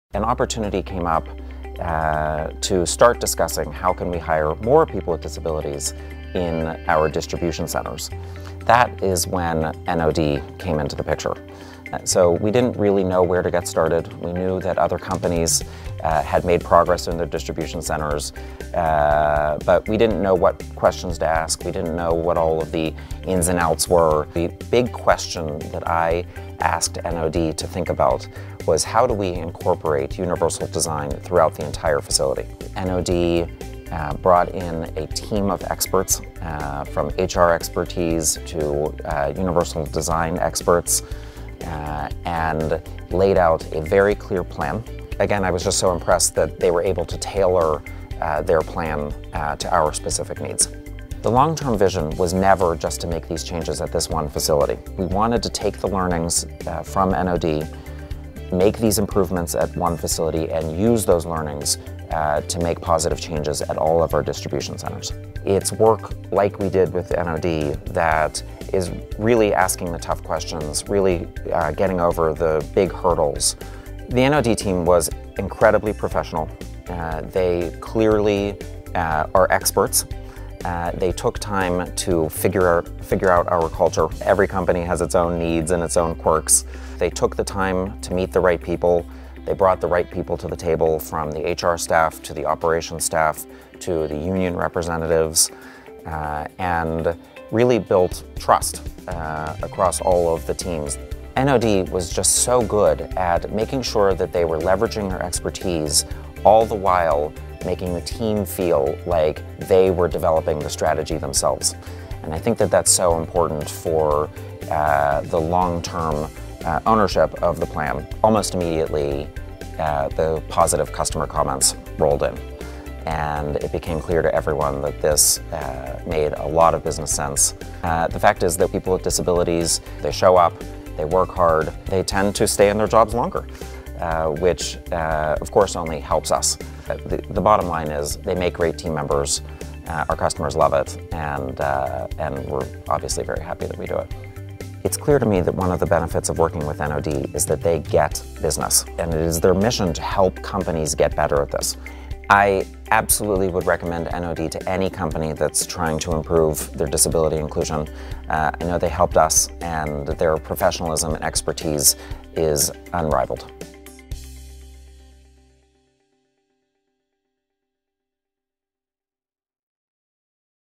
Giant Eagle + NOD | 'A customized workplace solution from the ground up'
Is your workplace a barrier to employing the best talent?

The grocery retailer Giant Eagle had an idea: gain an advantage in the competition for top talent by building a distribution center that works for all abilities.

Jeremy Shapira, Special Projects, Inclusion and Diversity at Giant Eagle explains how the National Organization on Disability developed a plan using universal design principles to meet the challenge, customized to his company's culture and goals.

With guidance from NOD's employment experts, Giant Eagle made it happen--and is seeing the additional benefits employees with disabilities offer for the return on investment.

Learn more about how NOD can help you company launch or enhance a disability inclusion initiative at NOD.org.